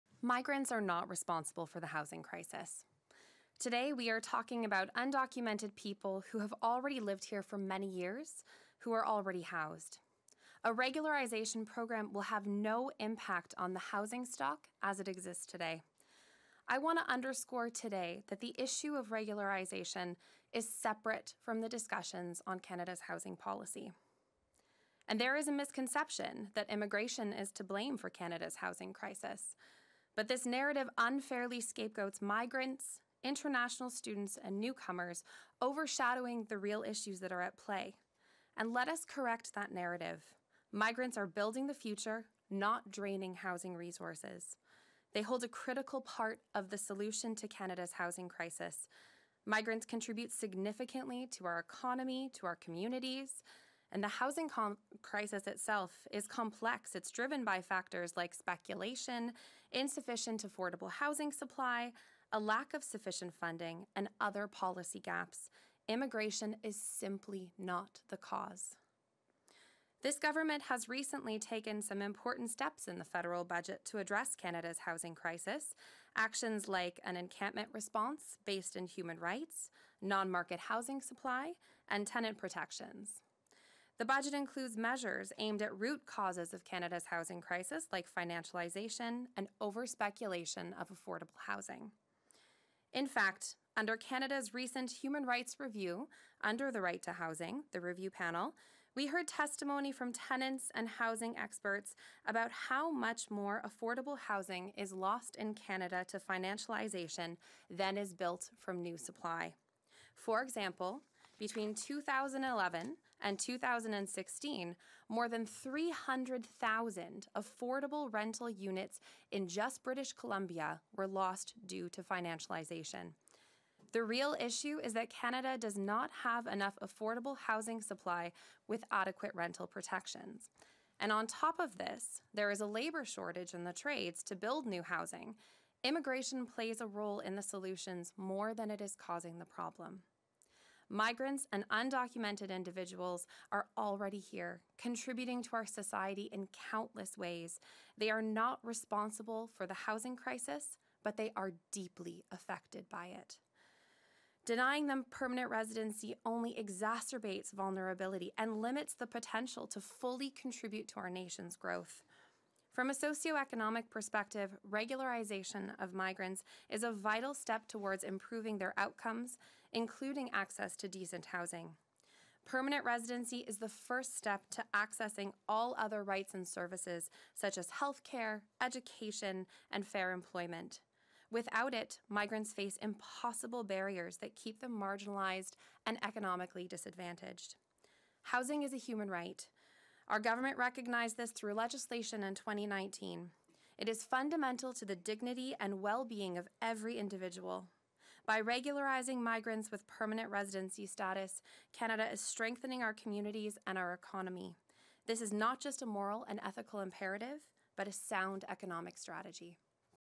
National Right to Housing Network - "Migrants are not to blame for the housing crisis"
Migrants are not to blame for the housing crisis. Regularizing undocumented people won't impact housing stock but will ensure fairness and equality.
Immigration isn’t the cause of the housing crisis; factors like speculation and lack of affordable housing are. Migrants contribute to our economy and deserve the same rights and protections.